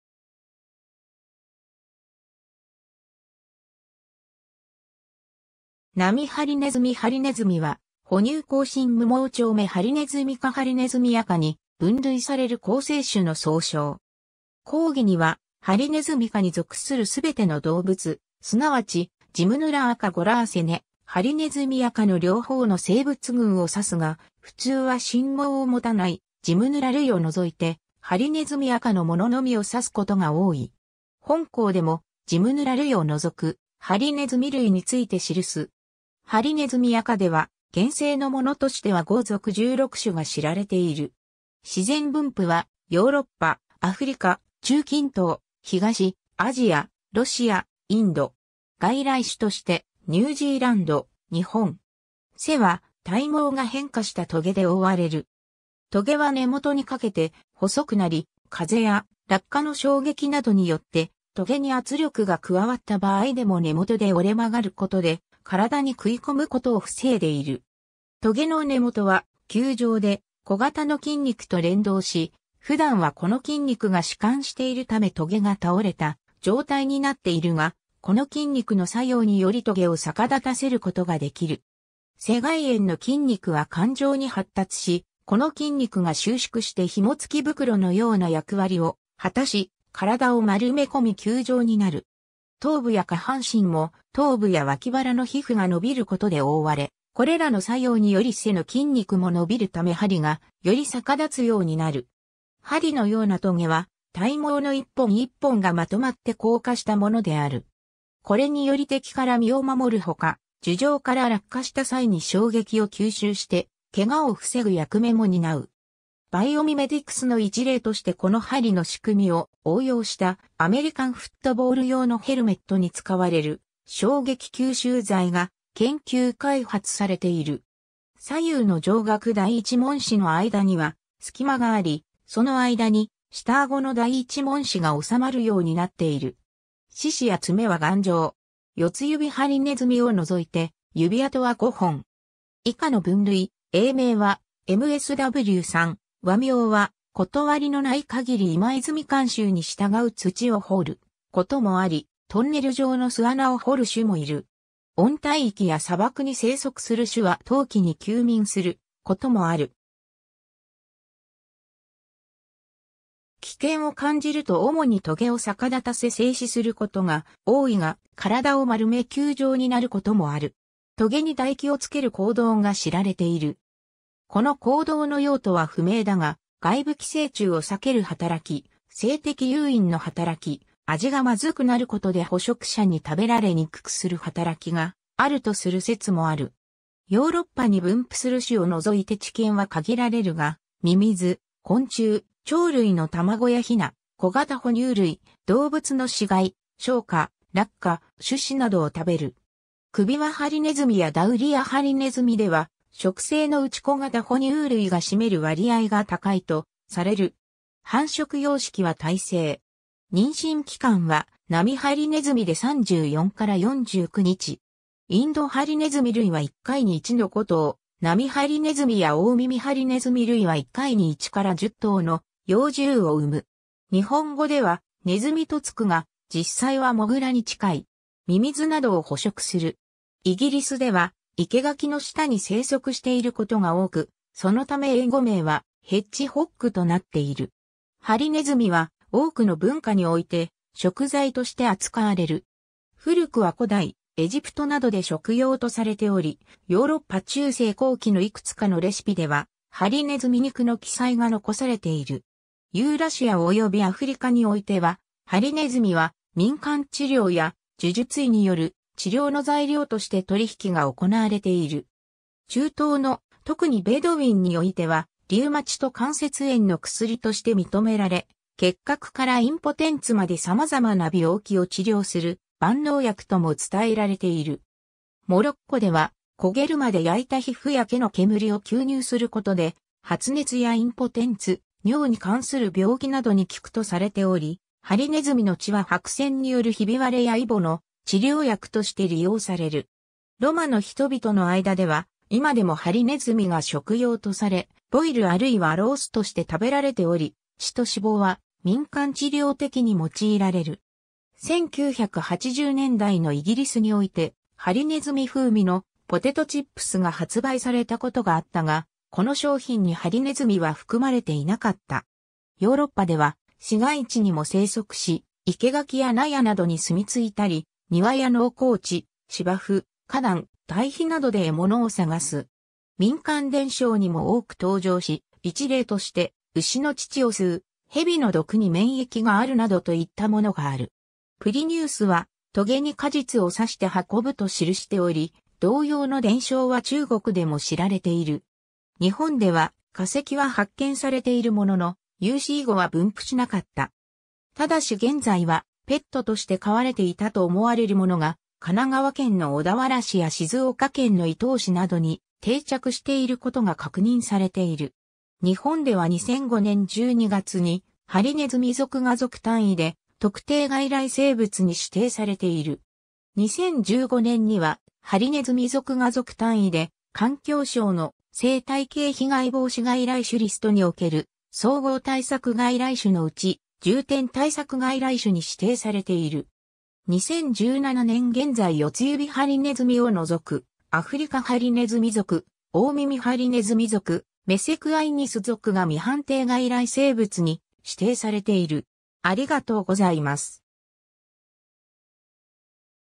ハリネズミ
ナミハリネズミ ハリネズミ（針鼠）は、哺乳綱真無盲腸目ハリネズミ科ハリネズミ亜科（Erinaceinae）に分類される構成種の総称。
 広義には、ハリネズミ科に属するすべての動物、すなわちジムヌラ亜科Galericinae、ハリネズミ亜科の両方の生物群を指すが、ふつうは針毛をもたない（あるいはほとんどもたない）ジムヌラ類を除いて、ハリネズミ亜科のもののみを指すことが多い。
本項でも、ジムヌラ類を除くハリネズミ類について記す。
ハリネズミ亜科では、現生のものとしては5属16種が知られている。
 自然分布は、ヨーロッパ、アフリカ、中近東、東アジア（日本を除く）、ロシア、インド。
 外来種として、ニュージーランド、日本。
 背は体毛が変化した棘で被われる。
棘は根元にかけて細くなり、風や落下の衝撃などによって棘に圧力が加わった場合でも根元で折れ曲がることで体に食い込むことを防いでいる。
棘の根元は球状で小型の筋肉と連動し普段はこの筋肉が弛緩しているため棘が倒れた状態になっているが、この筋肉の作用により棘を逆立たせることができる。
背外縁の筋肉は環状に発達し、この筋肉が収縮して紐付き袋のような役割を果たし体を丸めこみ球状になる。
頭部や下半身も頭部や脇腹の皮膚が伸びることで被われ、これらの作用により背の筋肉も伸びるため針がより逆立つようになる。
針のようなトゲは、体毛の一本一本がまとまって硬化したものである。
これにより敵から身を守る他、樹上から落下した際に衝撃を吸収して怪我を防ぐ役目も担う。
バイオミメティクスの一例としてこの針の仕組みを応用したアメリカンフットボール用のヘルメットに使われる衝撃吸収材が研究開発されている。
 左右の上顎第1門歯の間には隙間があり、その間に下顎の第1門歯が収まるようになっている。
四肢や爪は頑丈。
ヨツユビハリネズミ（後肢の趾が4本）を除いて指趾は5本。
 以下の分類・英名はMSW3(Hutterer, 2005)、和名は断りのないかぎり今泉監修(1986)に従う 土を掘ることもあり、トンネル状の巣穴を掘る種もいる。
温帯域や砂漠に生息する種は冬季に休眠することもある。
危険を感じると主に棘を逆立たせ静止することが多いが、体を丸め球状になることもある。
棘に唾液を付ける行動が知られている。
この行動の用途は不明だが外部寄生虫を避ける働き、性的誘因の働き、味がまずくなることで捕食者に食べられにくくする働きがあるとする説もある。
 ヨーロッパに分布する種を除いて知見は限られるが、ミミズ、昆虫、鳥類の卵や雛、小型哺乳類、動物の死骸、漿果、落果、種子などを食べる。
クビワハリネズミやダウリアハリネズミでは食性のうち小型哺乳類が占める割合が高いとされる。
 繁殖様式は胎生。
妊娠期間はナミハリネズミで34 - 49日。
インドハリネズミ類は1回に1 - 5頭、ナミハリネズミやオオミミハリネズミ類は1回に1 - 10頭の幼獣を産む。
 日本語では「ネズミ」と付くが、実際はモグラに近い。
ミミズなどを捕食する。
イギリスでは生垣の下に生息していることが多く、そのため英語名はHedgehog（生垣のブタ）となっている。
 ハリネズミは多くの文化において食材として扱われる。
古くは古代エジプトなどで食用とされており、ヨーロッパ中世後期のいくつかのレシピでは、ハリネズミ肉の記載が残されている。
ユーラシア及びアフリカにおいては、ハリネズミは民間治療や呪術医による治療の材料として取引が行われている。
中東の、特にベドウィンにおいてはリウマチと関節炎の薬として認められ、結核からインポテンツまでさまざまな病気を治療する万能薬とも伝えられている。
 モロッコでは、焦げるまで焼いた皮膚や毛の煙を吸入することで、発熱やインポテンツ、尿に関する病気などに利くとされており、ハリネズミの血は白癬によるひび割れやイボの治療薬として利用される。
ロマの人々の間ではいまでもハリネズミが食用とされ、ボイルあるいはローストして食べられており、血と脂肪は民間治療的に用いられる。
1980年代のイギリスにおいて「ハリネズミ風味」のポテトチップスが発売されたことがあったが、この商品にハリネズミは含まれていなかった。
 ヨーロッパでは市街地にも生息し、生け垣や納屋などに住み着いたり庭や農耕地・芝生・花壇・堆肥などで獲物を探す。
民間伝承にも多く登場し、一例としてウシの乳を吸う・ヘビの毒に免疫があるなどといったものがある。
Pliniusは「棘に果実を刺して運ぶ」と記しており、同様の伝承は中国で...